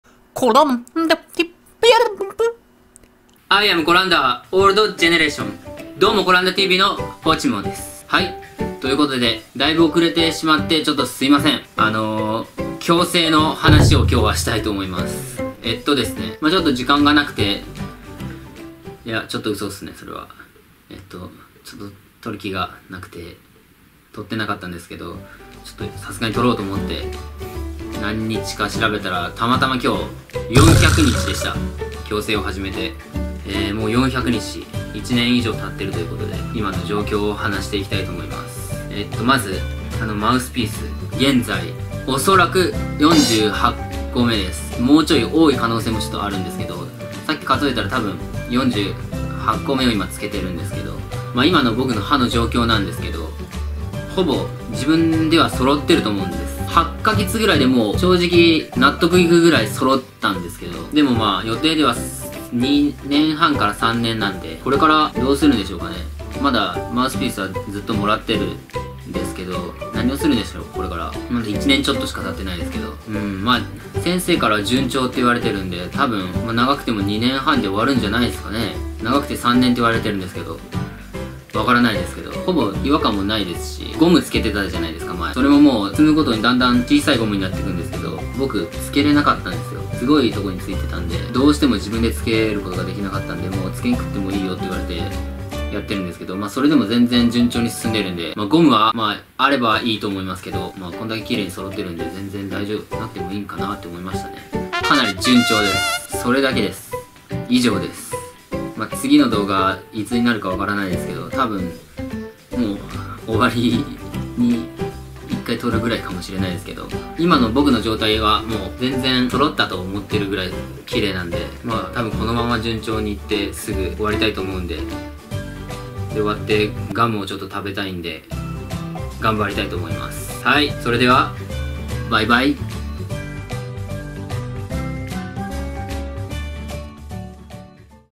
【矯正400日目】ガムを食べずに生きてきた！
149本目の動画です。

はい！久しぶりのポチモ矯正状況報告になります。
もう半年経ったと考えると早いものですね。

現在もまだ続けていていつまでかかるのかも分かりません。

根気強く頑張っていきたいと思います。

【おすすめ動画①】→【第１回】メンバーの考えは一緒なのか!?”全員一致ゲーム”